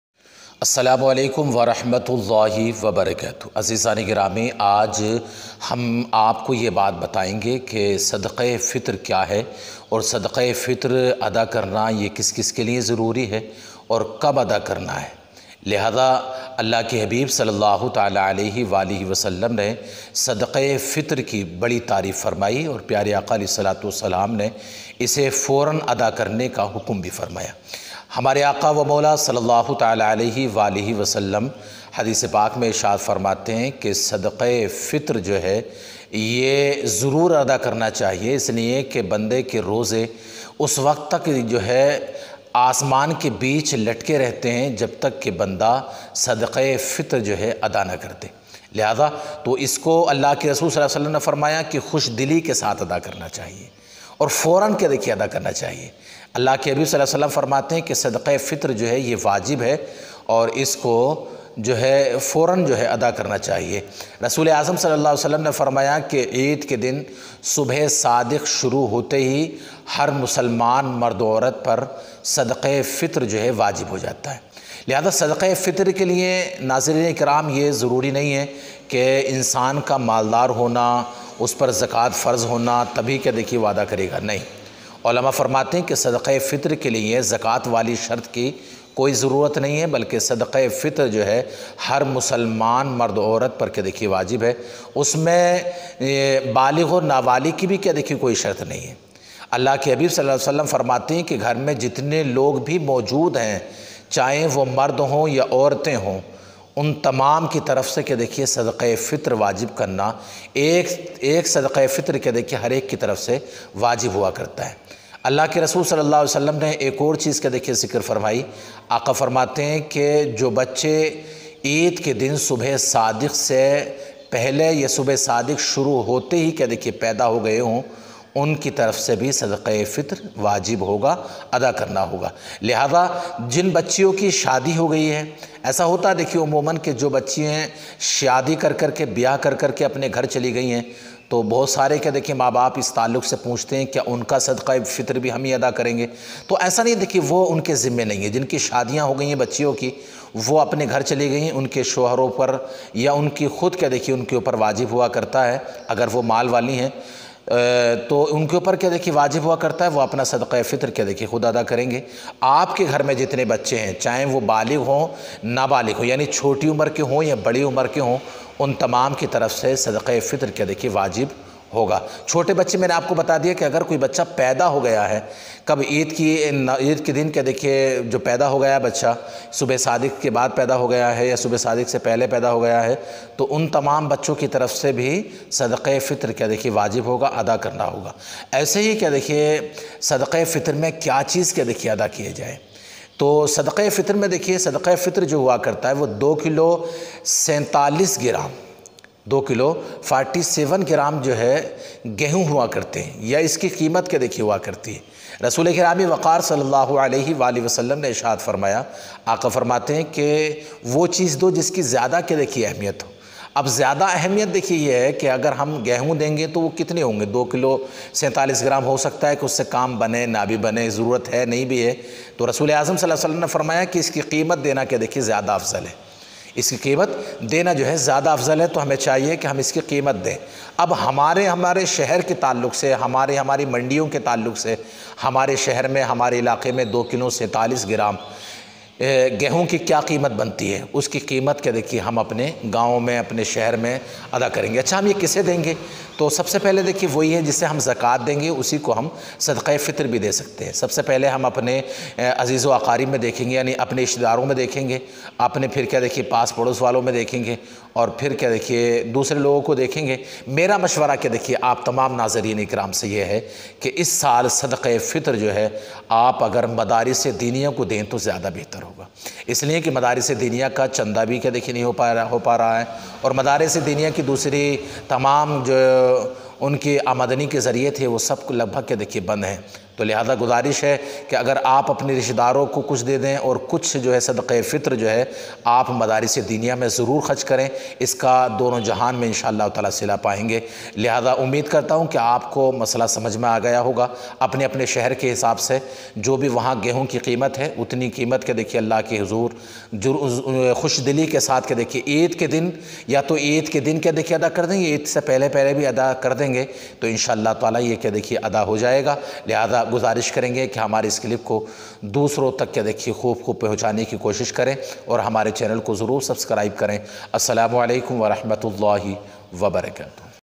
Sadqa e Fitr Kese Ada Kare । सदका ए फित्र अदा करने का मुकम्मल तरीका । by Molana Siraj Ali Razvi 2021
Sadqa e Fitr Kese Ada Kare । सदका ए फित्र अदा करने का मुकम्मल तरीका.
                                            "Aaj Hamne Is Video Main Poori tafseel ke sath bataya hai